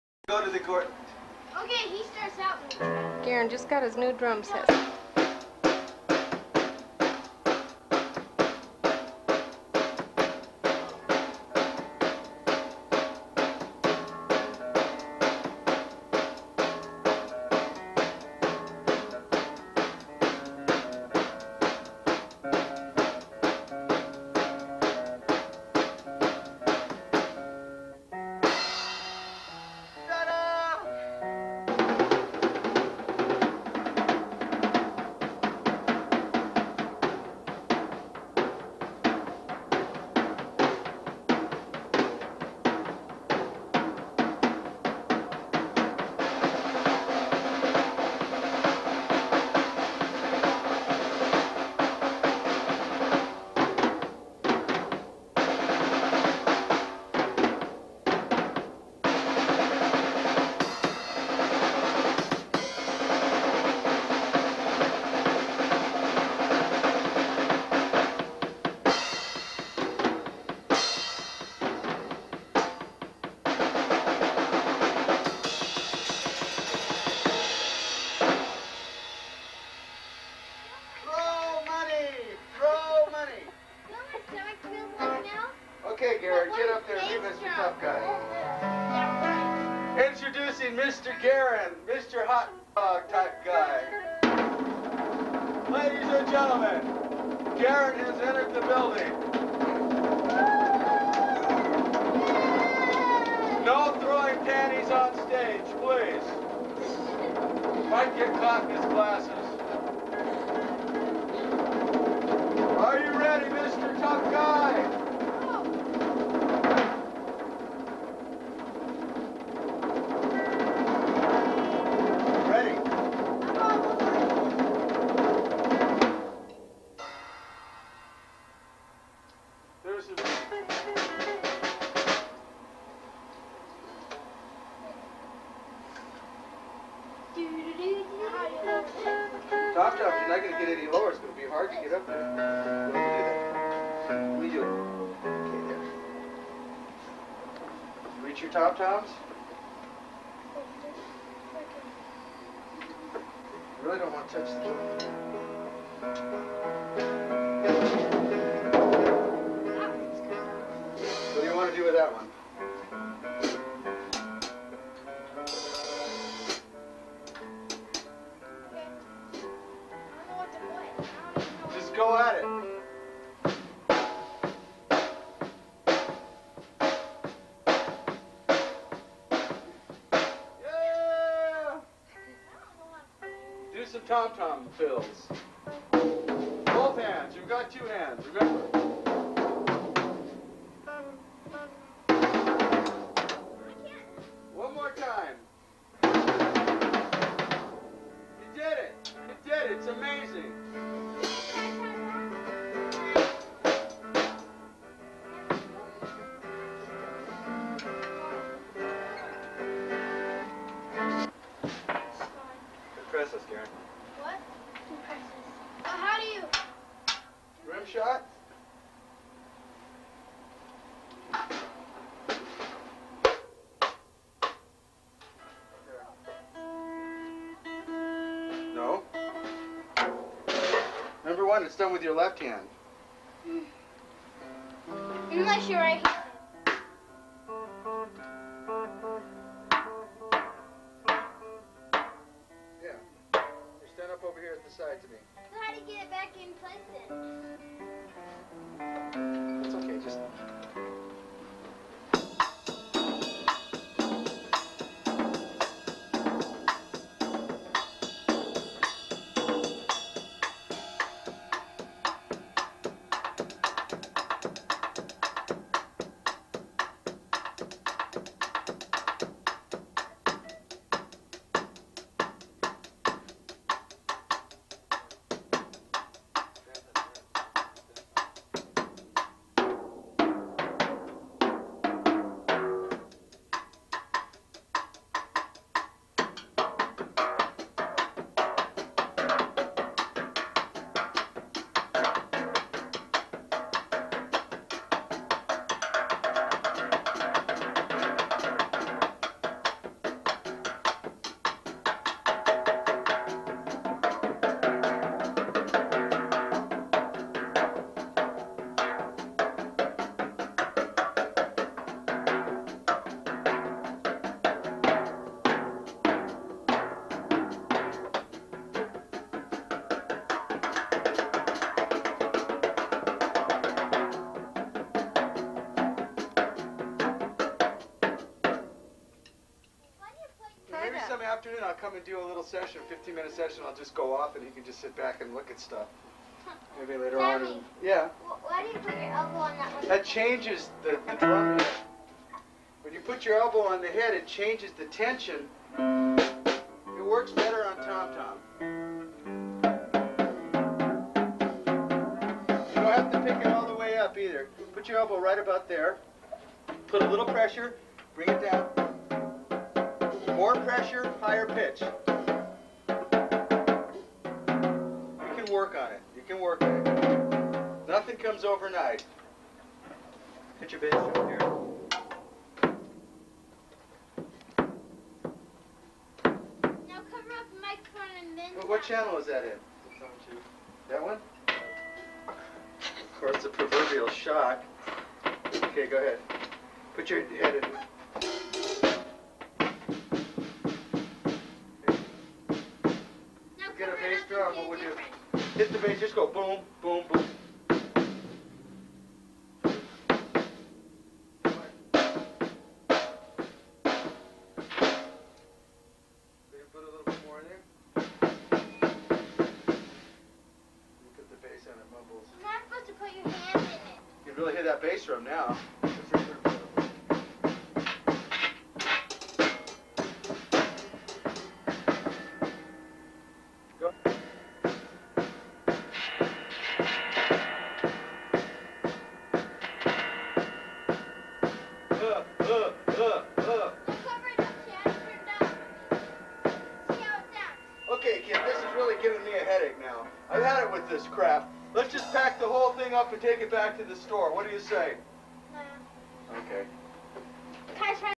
New drum set and drum lesson with Sammy 1993
Garrin got a new Pearl drum set and his instructor Sammy helped him set it up and gave him a lesson at our house.